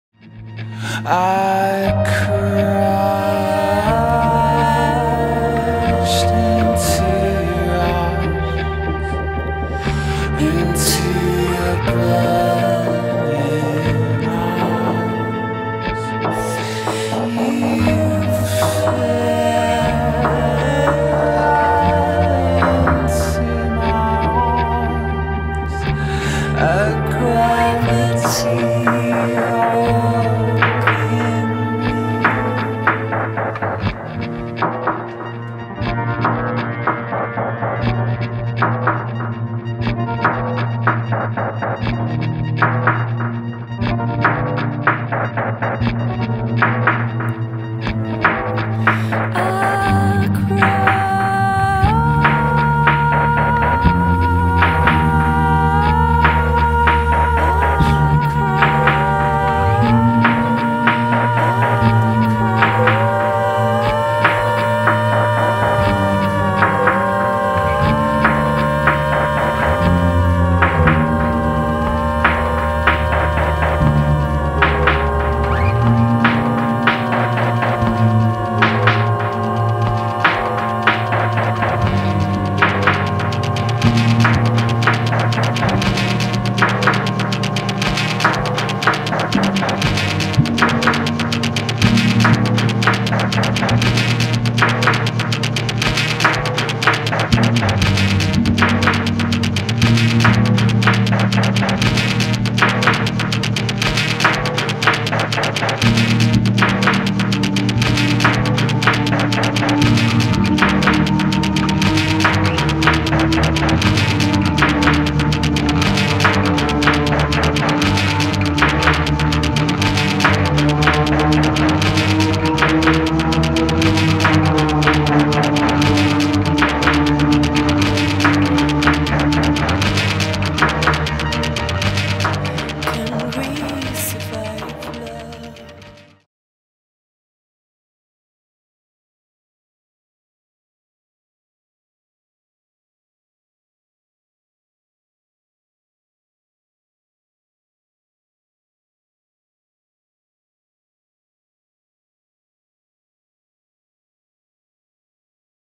Intense Attachment Cast Doncaster
Intense Attachment Cast Doncaster 4K. An intimate dance theatre work ecplofomg parenthood, identity and attachment. This is one of three works being made over nine years.